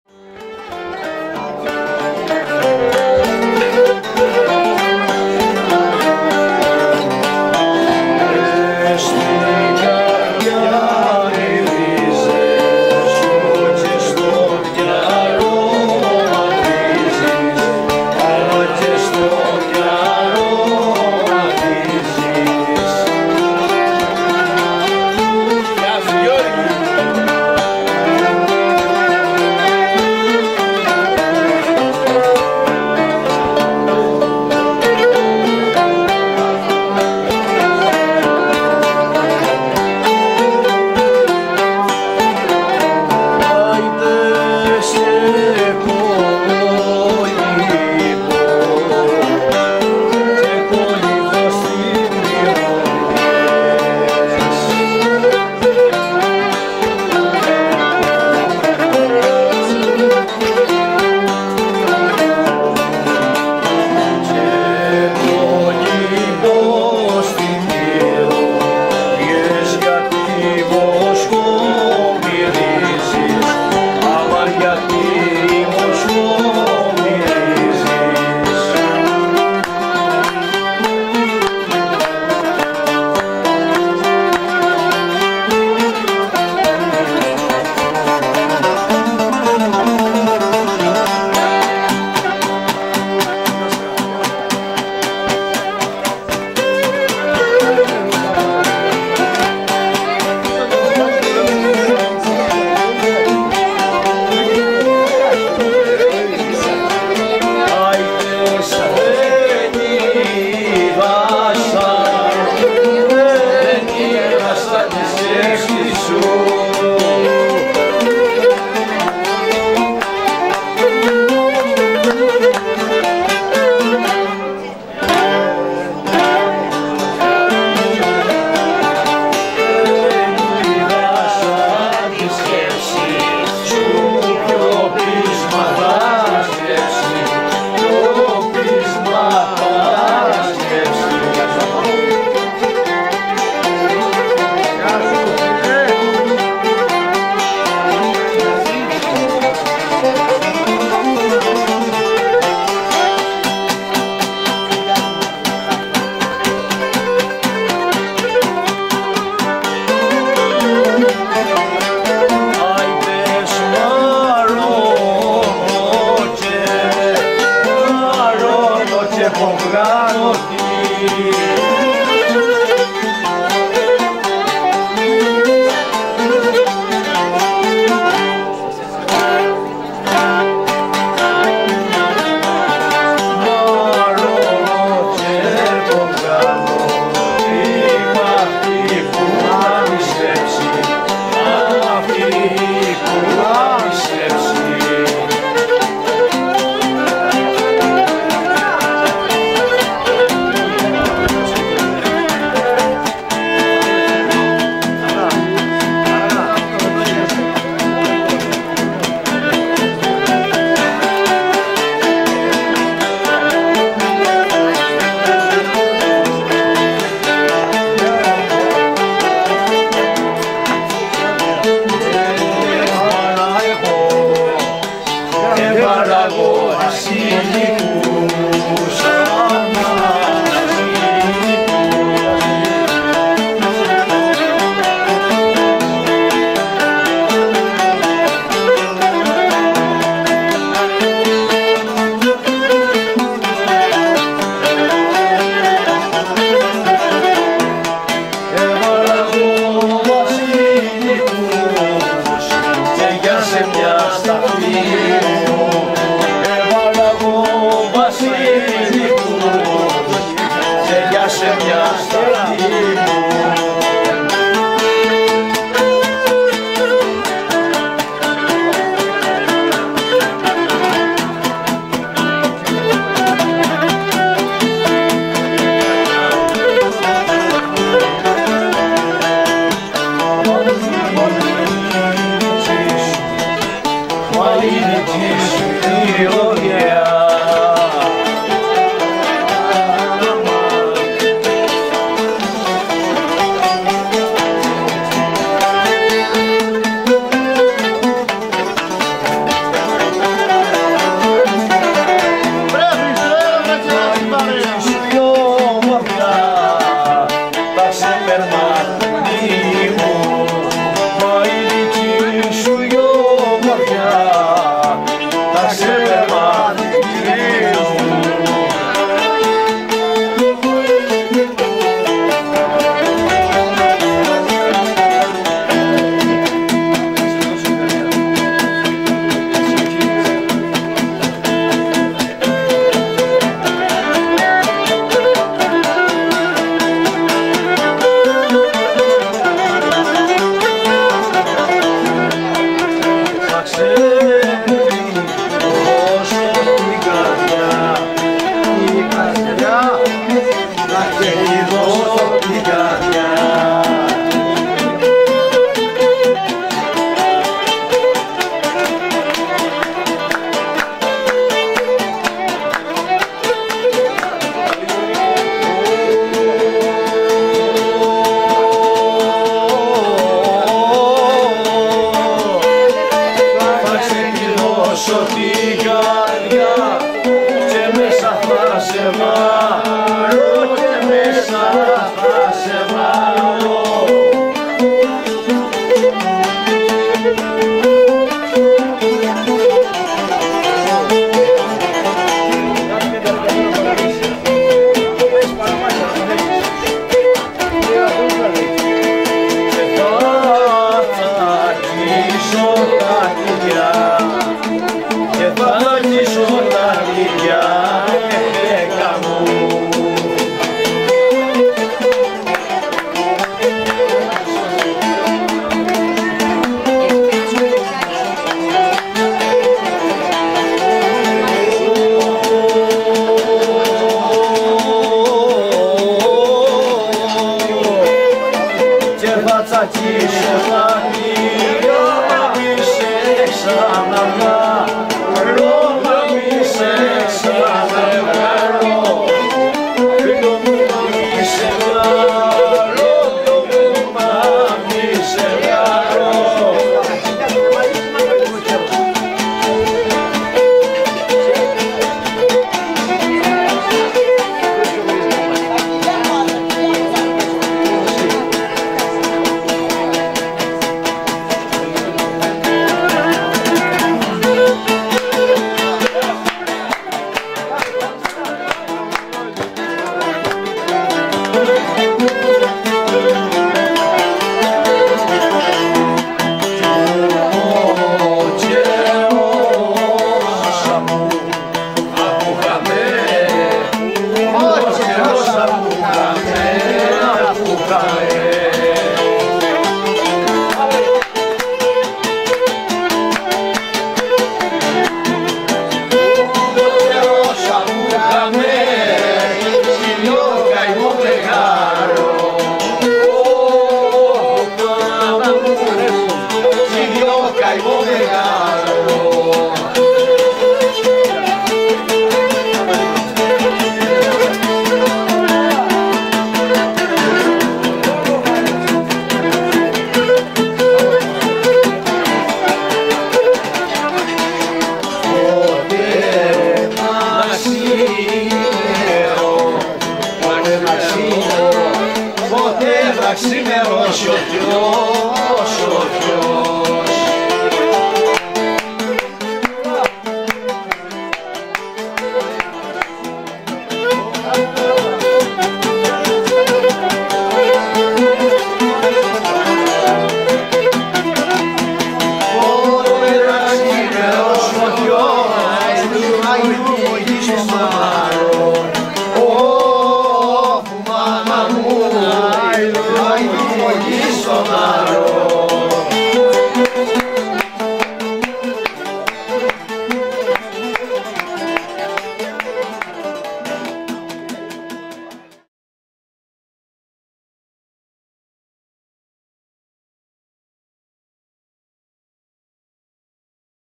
Αντώνης & Μανώλης Χαριτάκης παρέα με Πολογιάννη συρτά
Με την κάμερα του Αντώνη Κουκλινού στη βάφτιση του Στέλιο Ξυλούρη στο ''Πέτρινο'' 10 Σεπτέμβρη 2015